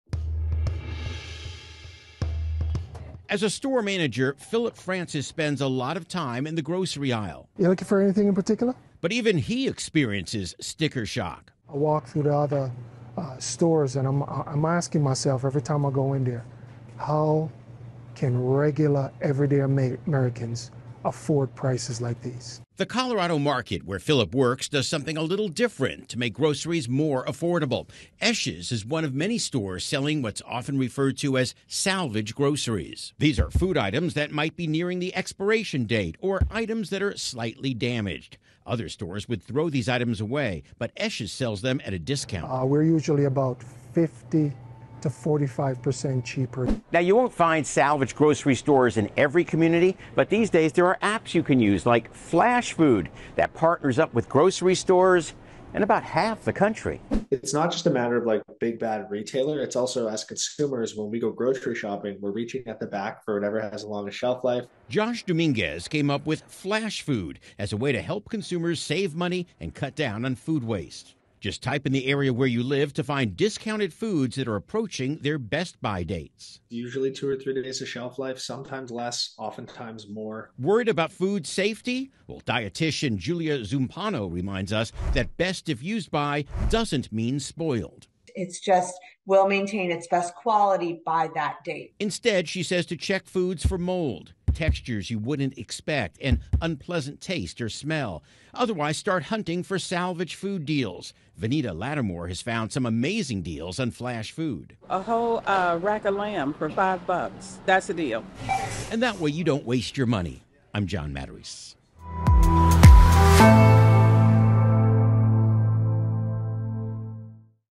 Salvage grocery stores booming as food prices soar
As Americans face rising prices, business is booming at salvage grocery stores.

Places like Esh's, a market in Colorado, do things differently to make groceries more affordable. Among all the items they sell, some items might be nearing their expiration date. In some cases, packages are slightly damaged.

Other stores would often throw the items away, but salvage stores sell them at a discount.

"We're usually about 50-45% cheaper than any store out there," said Phillip Francis, a manager at Esh's.